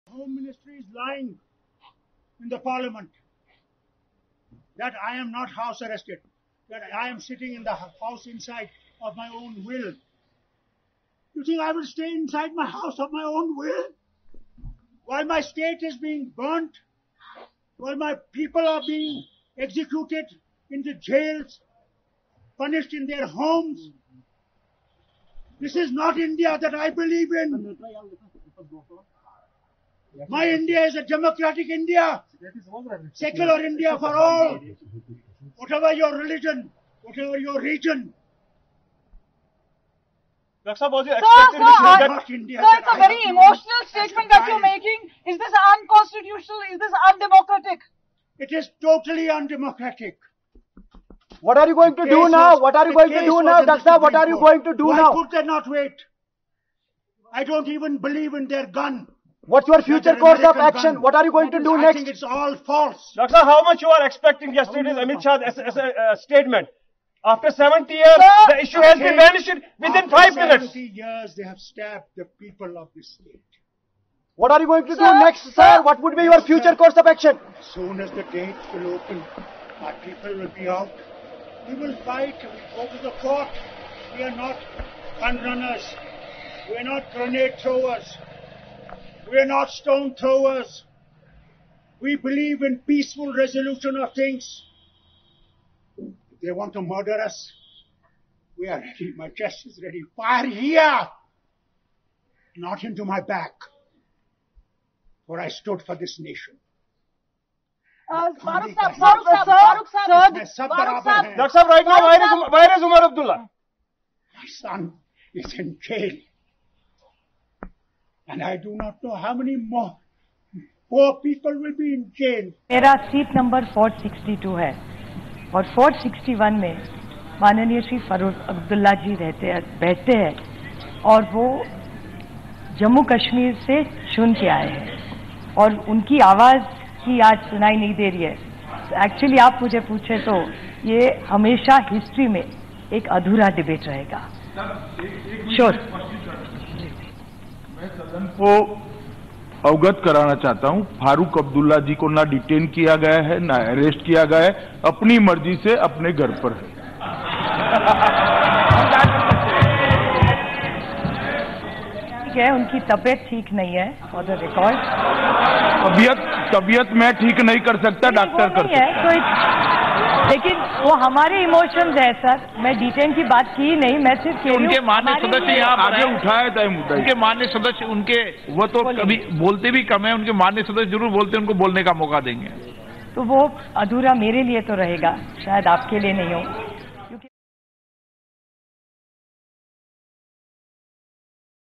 Farooq Abdullah cries house arrest, HM Amit Shah denies in Lok Sabha
Jammu and Kashmir National Conference leader Farooq Abdullah claimed on Tuesday that he had been placed under house arrest. He said that as soon as he is released, he would stage a legal counter to the abrogation of Article 370, which ended the special status accorded to Jammu and Kashmir. His absence in Lok Sabha had earlier been raised by Nationalist Congress Party MP Supriya Sule. In response, Union Home Minister Amit Shah claimed that Abdullah was not under house arrest, but had skipped Parliament of his own volition.